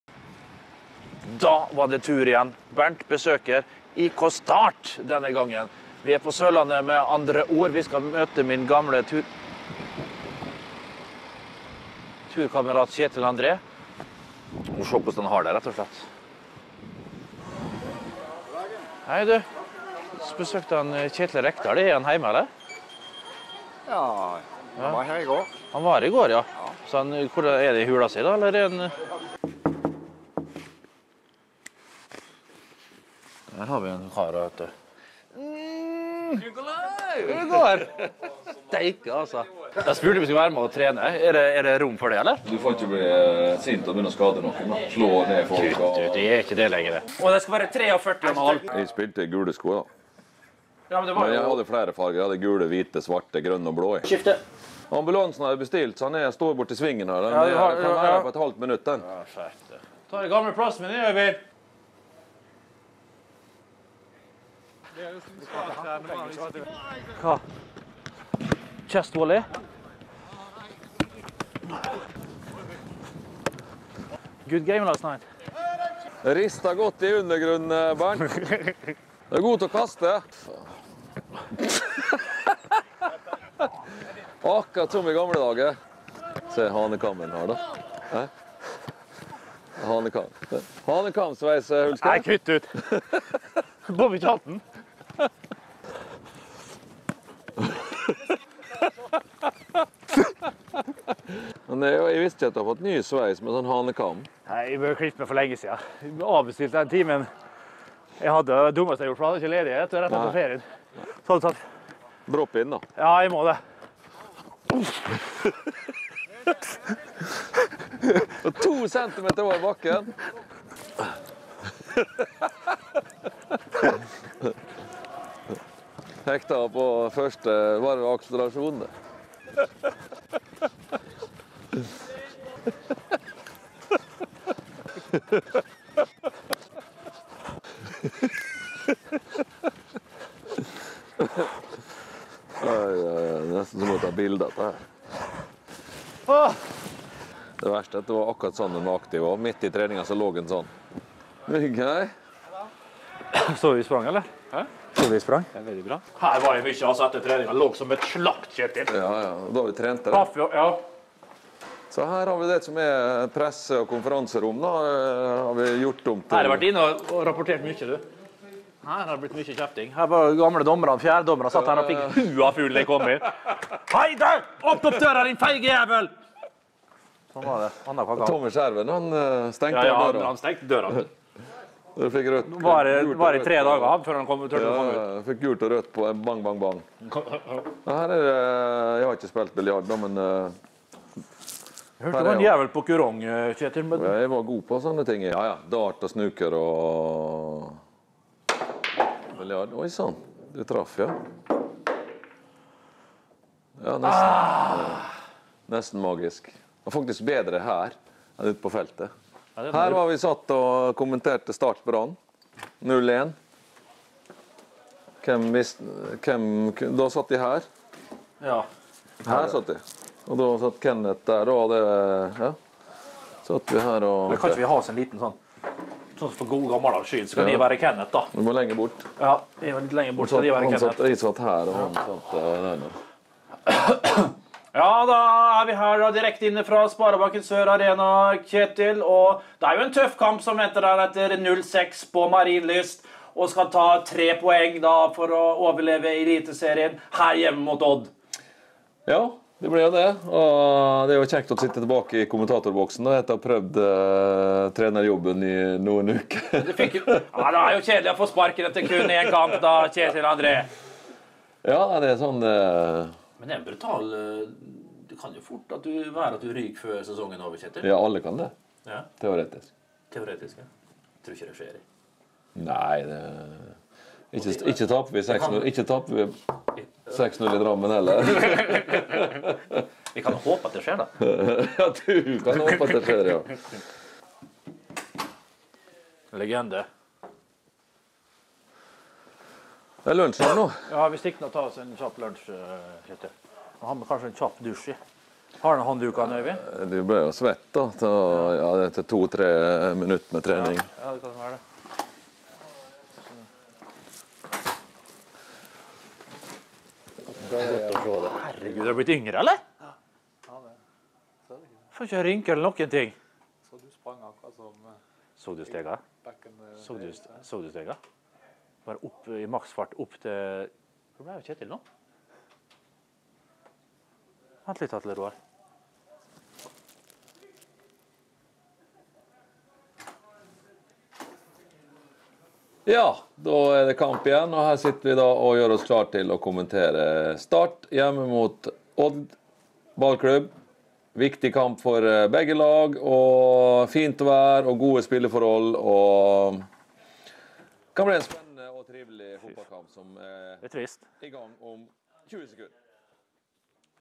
Rekdal knekker sammen av Hulsker på Start-trening
Bernt Hulsker er og besøker sin gamle reisekamerat og venn i Start. Kjetil Rekdal begynner å bli varm i trenerstolen på Sørlandet og lar sin tidligere elev får delta på treningen på Sør Arena.